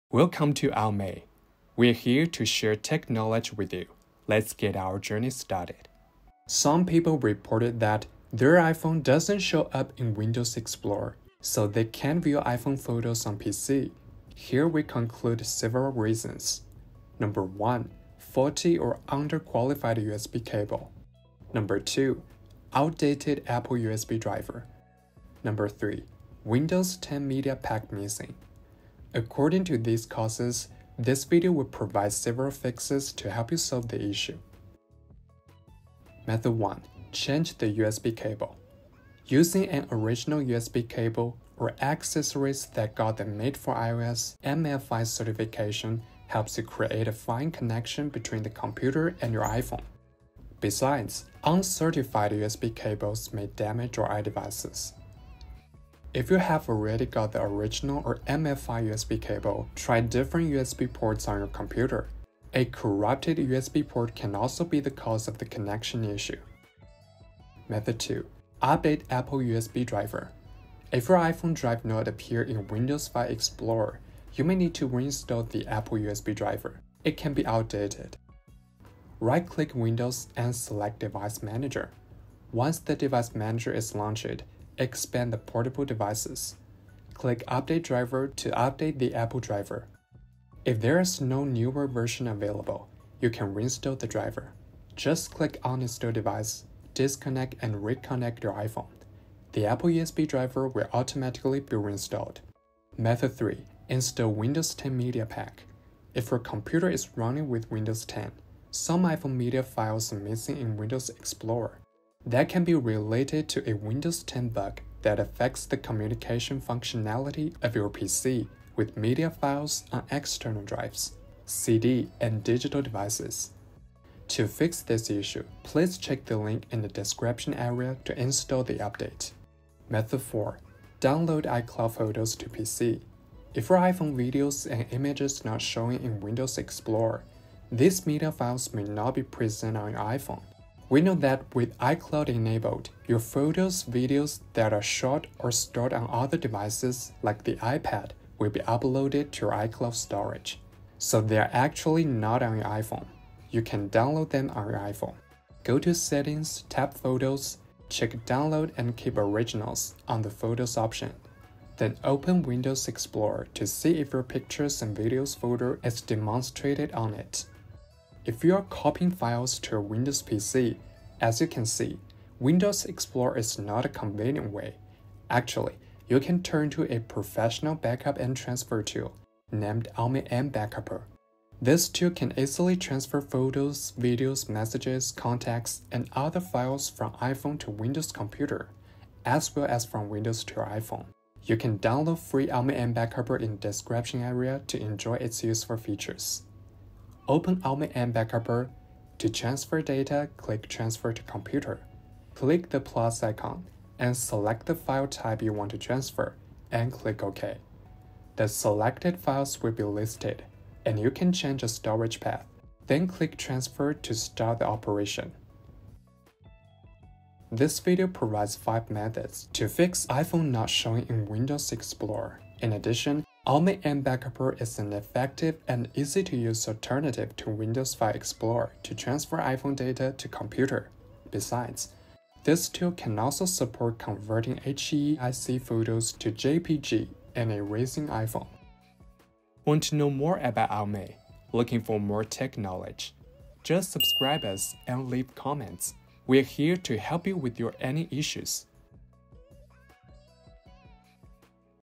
[Solved] iPhone Not Showing in Windows File Explorer
🔔 Download Free FoneTool Now (former name: AOMEI MBackupper)

When you want to manage and transfer media files from iPhone to Windows PC, you may find your iPhone not showing in Windows Explorer. There are different reasons that may lead to this issue. Check out this video and get the solution that suits you.

✅ 【iPhone Not Showing in Windows 11/10/8/7 File Explorer】
⏰ 00:00 Start
⏰ 00:36 Method 1. Change the USB Cable
⏰ 01:08 Method 2. Update Apple USB Driver
⏰ 01:43 Method 3. Install Windows 10 Media Pack
⏰ 02:10 Method 4. Download iCloud Photos to PC
⏰ 02:56 Method 5. AOMEI MBackupper

✅ 【Detail】
⏰ 00:36 Method 1. Change the USB Cable
Using an original USB cable or accessories that got the Made For iOS(MFi) certification helps you create a fine connection between the computer and your iPhone. Besides, uncertified USB cables may damage your iDevices.
If you have already got the original or MFi USB cable, try different USB ports on your computer. A corrupted USB port can also be the cause of the connection issue.

⏰ 01:08 Method 2. Update Apple USB Driver
If your iPhone not appearing in file explorer, you may need to reinstall the Apple USB driver. It can be outdated.
1. Right-click Win and select Device Manager.
2. Once the Device Manager is launched, expand the “Portable Device”.
3. Click “Update Driver” to update the Apple driver.

⏰ 01:43 Method 3. Install Windows 10 Media Pack
If your computer is running with Windows 10, some iPhone media files are missing in Windows Explorer. That can be related to a Windows 10 bug that affects the communication functionality of your PC with media files on external drives, CDs, and digital devices.

⏰ 02:10 Method 4. Download iCloud Photos to PC
If your iPhone videos and images not showing in Windows Explorer, these media files may not be present on your iPhone. We know that with iCloud enabled, your photos and videos that are shot or stored on other devices like the iPad will be uploaded to your iCloud storage. So they are actually not on your iPhone, you can download them on your iPhone.
1. Go to the “Settings”. Tap “Photos”.
2. Check “Download and Keep Originals” on the Photos option.
Then open Windows Explorer to see if your pictures and videos folder is demonstrated on it.

⏰ 02:56 Method 5. AOMEI MBackupper
If you want to copy files to your Windows PC, as you can see, Windows Explorer is not a convenient way, actually, you can turn to a professional backup & transfer tool named AOMEI MBackupper. This tool can easily transfer photos, videos, messages, contacts, and other files from iPhone to Windows computer, as well as from Windows to your iPhone.

1. Open AOMEI MBackupper. To transfer data, click “Transfer to Computer”.
2. Click the “+” icon and select the file type you want to transfer, and click “OK”.
3. The selected files will be listed, and you can change a storage path. Then click “Transfer” to start the operation.